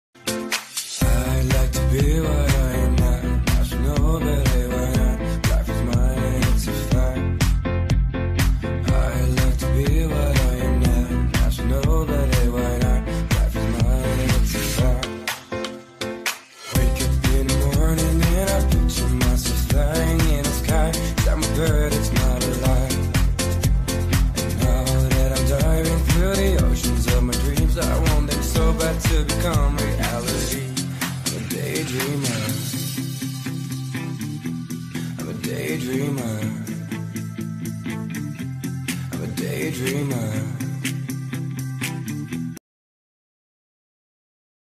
Retractable dining table with induction cooker
Retractable smart dining table, induction cooker, heat preservation turntable, storage box,marble/rock surface.